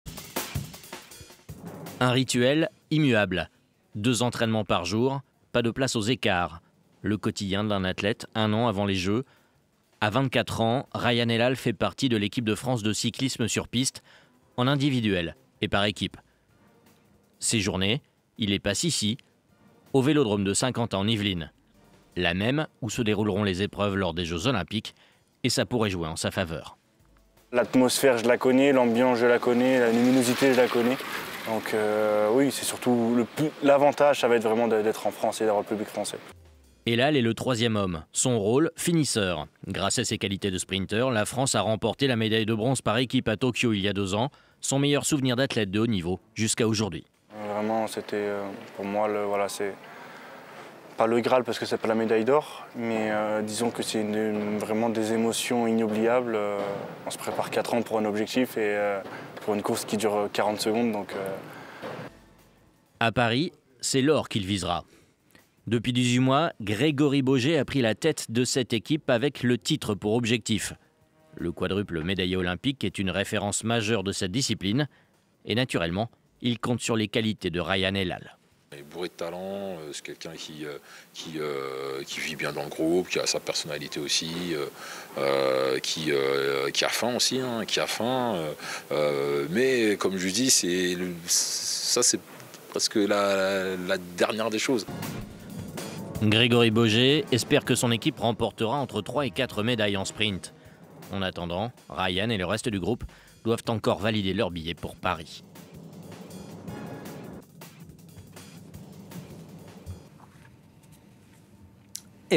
Cyclisme sur piste : Rayan Helal, la fusée du sprint avec objectif Paris-2024 • FRANCE 24
Médaillé de bronze avec l'équipe de France de vitesse lors des JO de Tokyo, le cycliste Rayan Helal espère se hisser sur la plus haute marche du podium lors des Jeux olympiques de Paris. Le pistard met tout en œuvre pour maximiser ses chances de qualification.